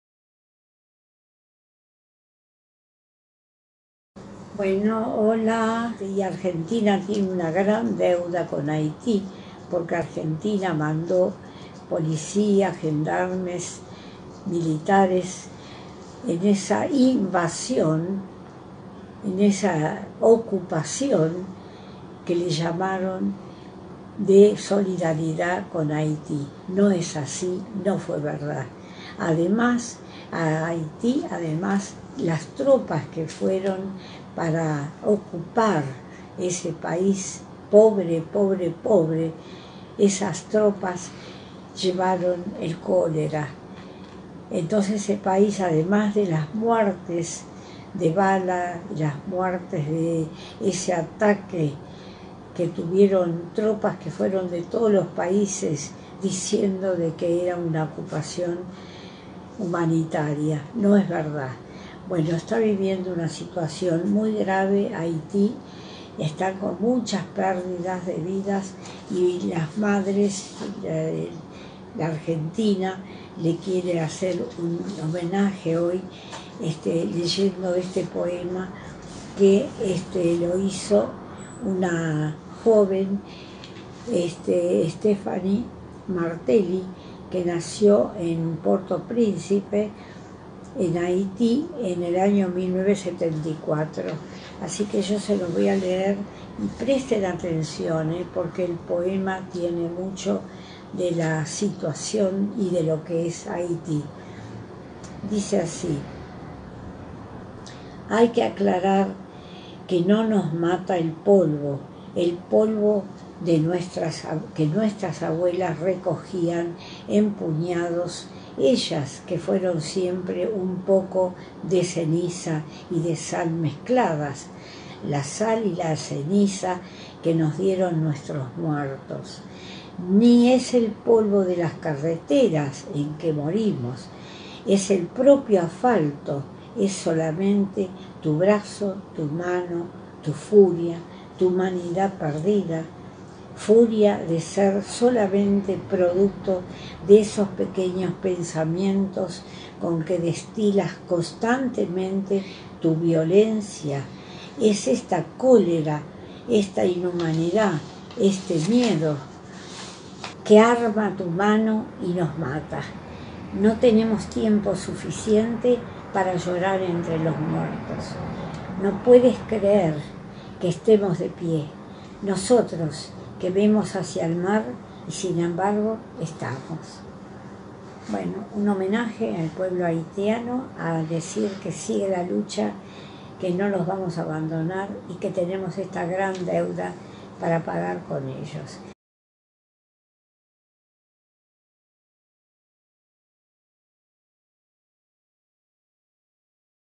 Nuestra deuda con Haití
Nora Cortiñas de Madres de Plaza de Mayo-Línea Fundadora, comparte sobre la deuda que desde Argentina se tiene con el pueblo de Haití, en momentos de fuerte crisis y movilización en aquel país que llama a nuestra solidaridad. Invita a acompañar a los movimientos populares haitianos en la CONFERENCIA INTERNACIONAL a realizarse el viernes, 29/1, a las 15 hs en Argentina (UTC-3). La cita es por zoom